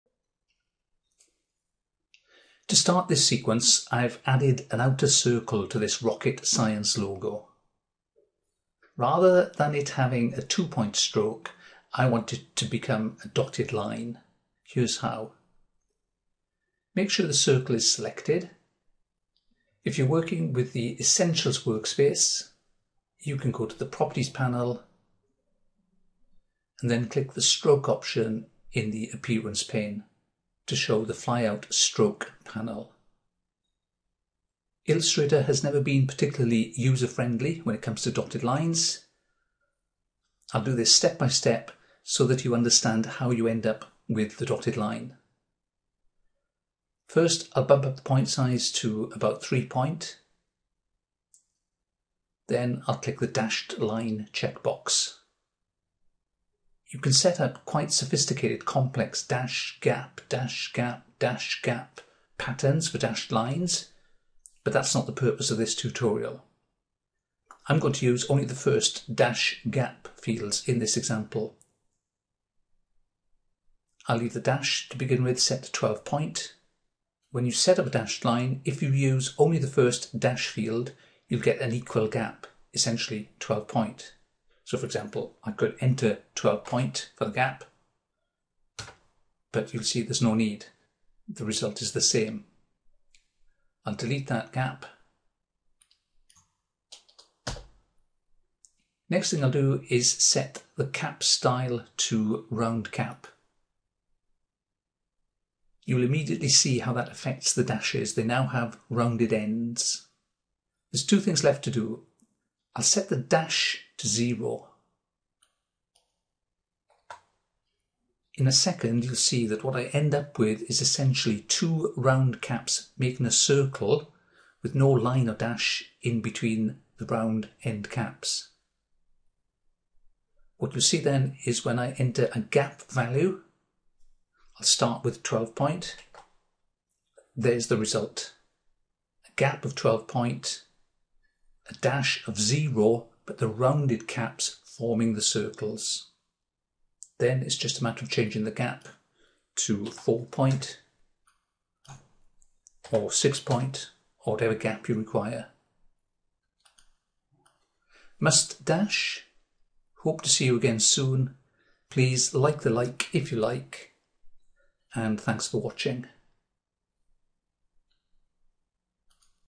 How to created dotted lines using Adobe Illustrator's Stroke panel
How to create a dotted line in Adobe Illustrator using Round Cap styles and dash gap patterns in the Stroke panel. For onsite Illustrator training in the UK